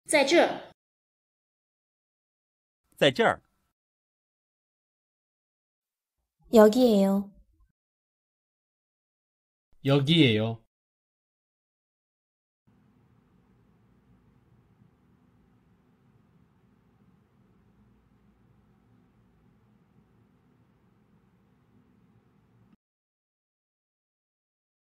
중국어(전통)로 "여기예요." 말하는 법
남자 목소리 :
在這兒！

여성 목소리 :
在這兒！

남성 음성 번역 :
여기예요.

여성 음성 번역 :
여기예요.